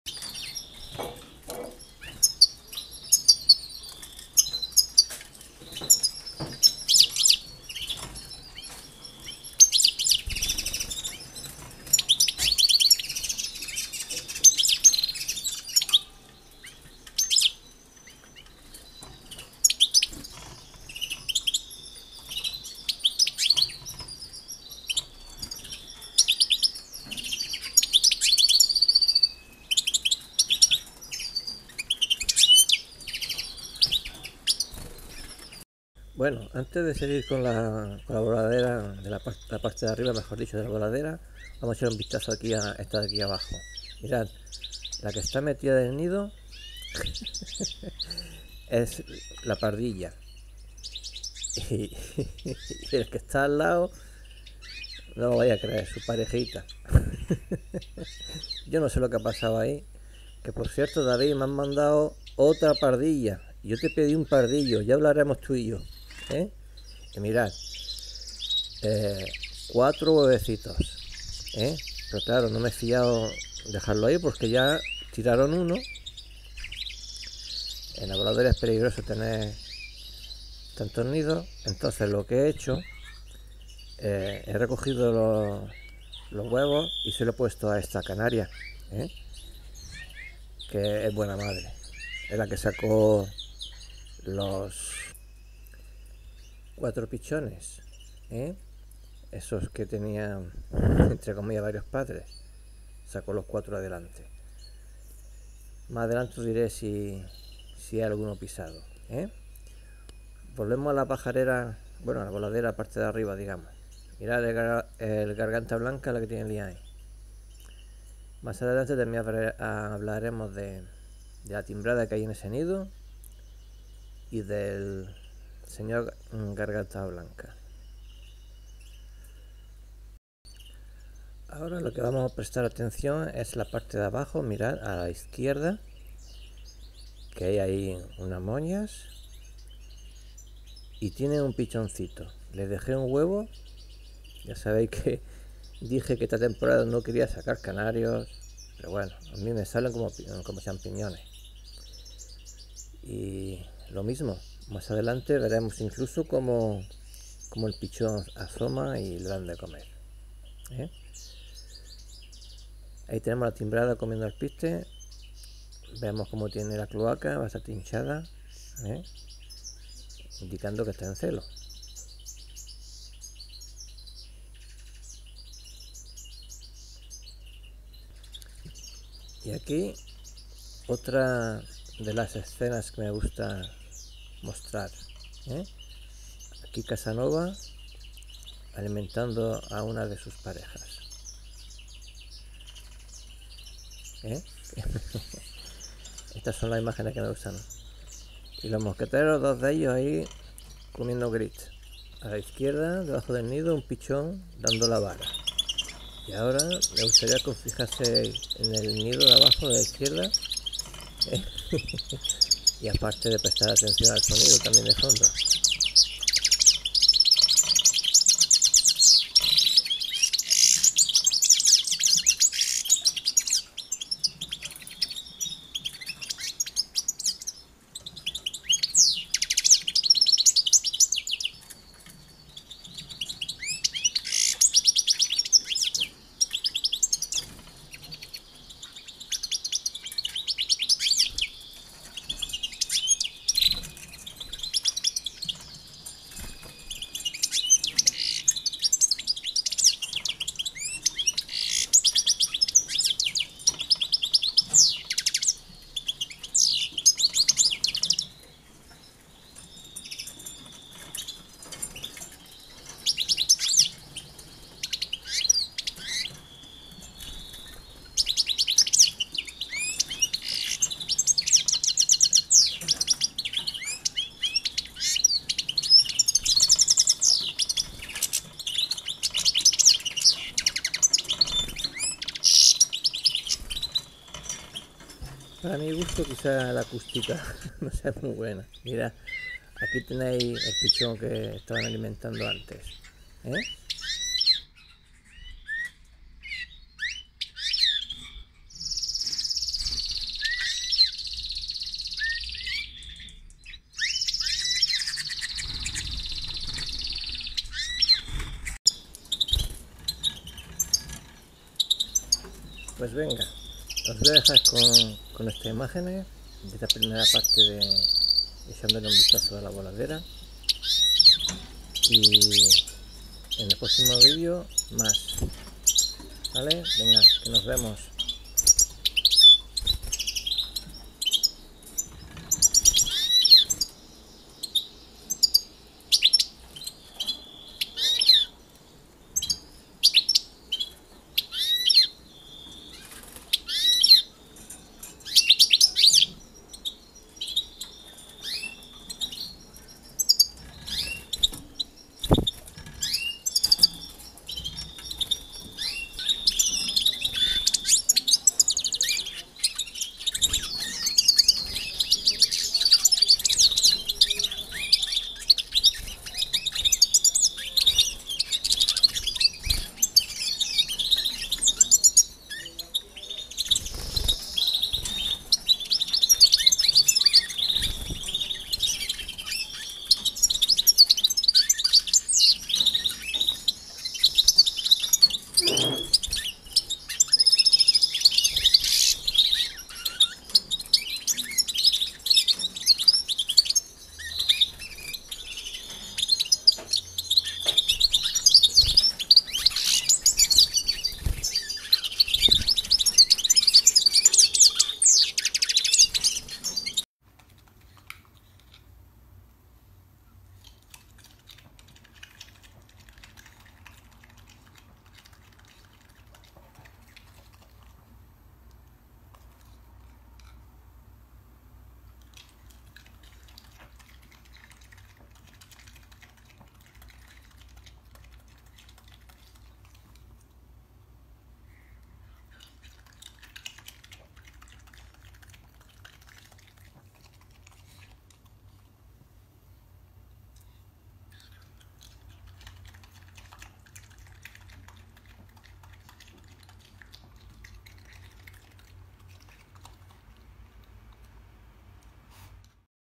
El Aviario De Loki - Un Rato En La Voladera Part 1
Compañer@s un rato en la voladera y os perdais el final del video que ahi os enseño como se debe comer la fruta.